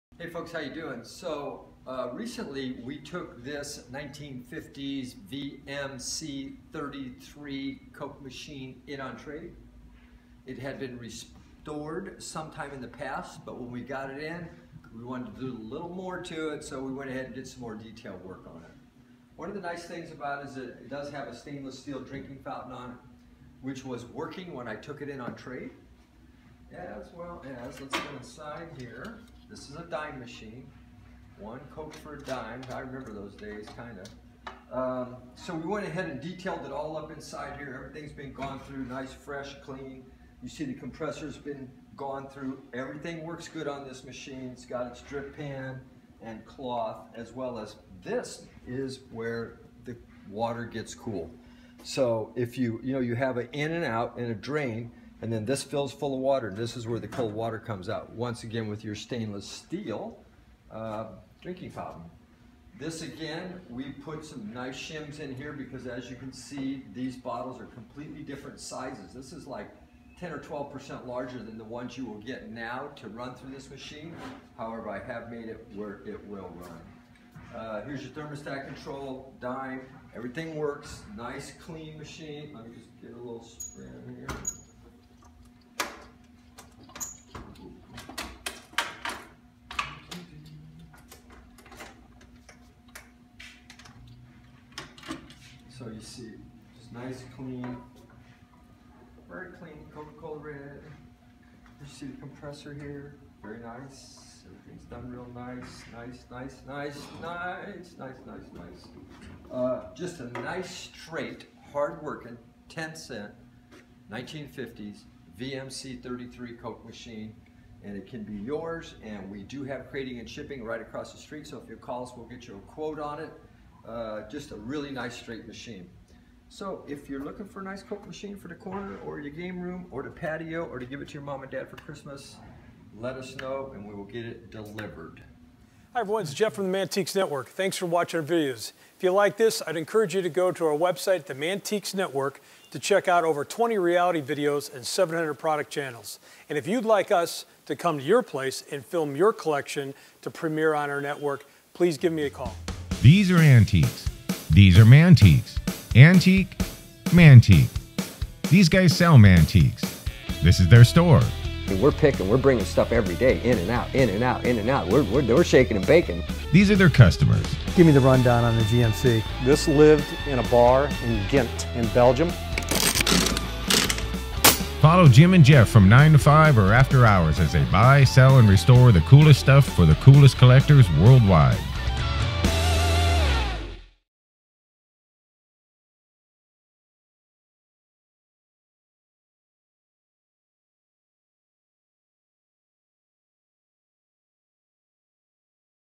1950's VMC 33 Coke Vending Machine w/ Water Fountain SOLD
1950's VMC 33 Coke Vending Machine w/ Water Fountain For Sale $4,995

eBay Listing

The Mantiques Network is the ultimate shopping experience and social network for the vintage collector and enthusiast.